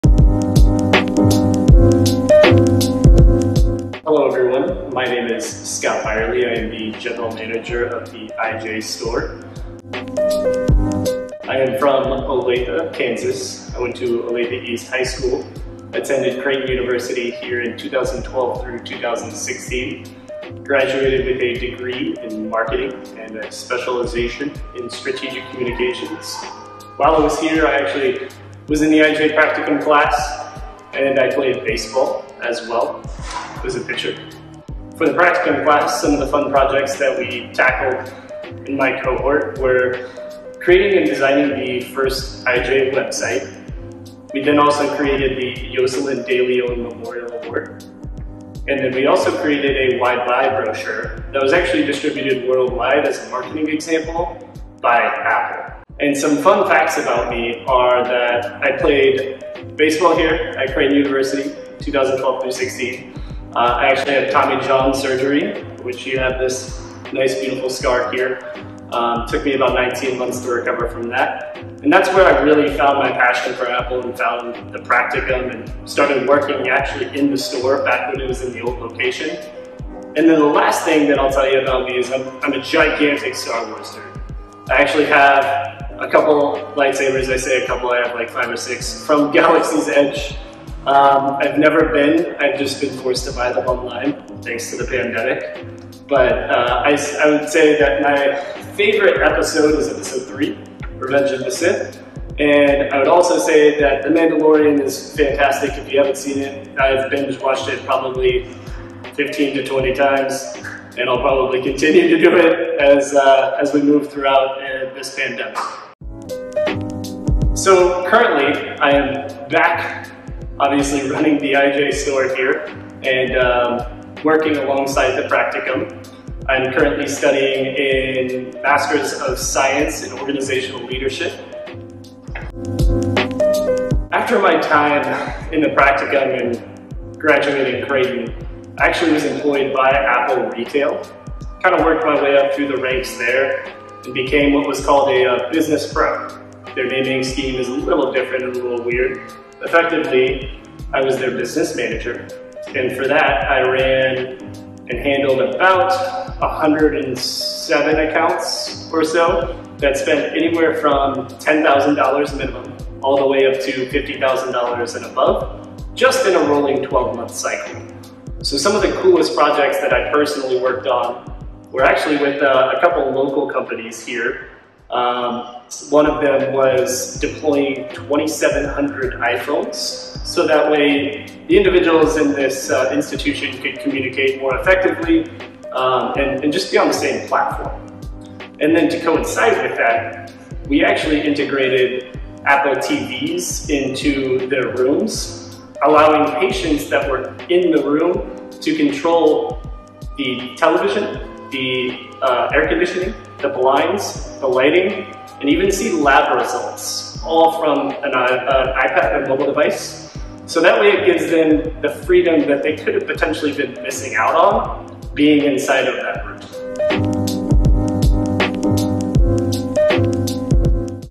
Meet the Manager | iJay
You have seen him in many of our JayCuts episodes, so now it is time to learn more about our store manager, Scott! He is a Creighton grad and is going to be sharing his experience with the iJay Practicum as well as his life after college working for Apple.